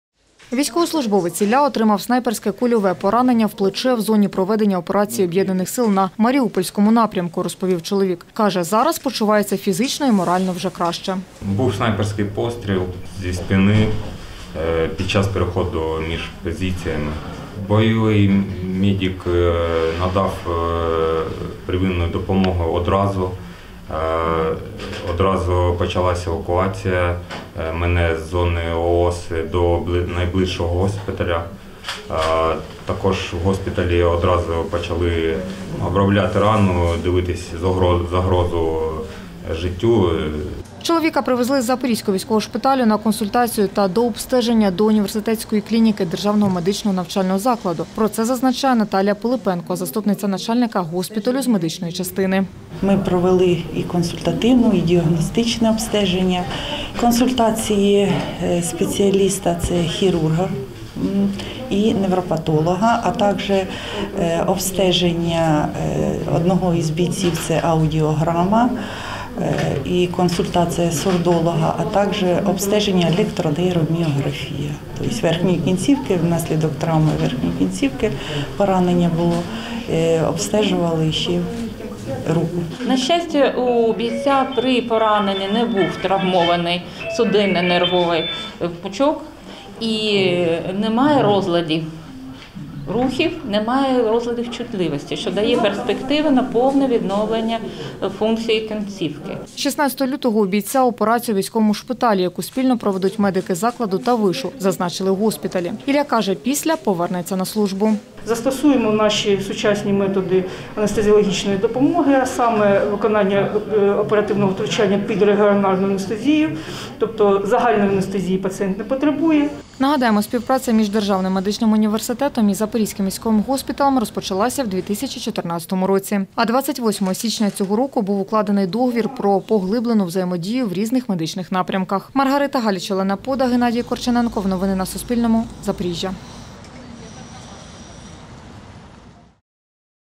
Університетська клініка ЗДМУ лікує поранених бійців | Новини
Військовослужбовця зі снайперським пораненням привезли на консультацію до університетської клініки Запорізького державного медичного університету. В закладі пояснюють, цьогоріч — це вже не вперше, бійців привозять з різними травмами.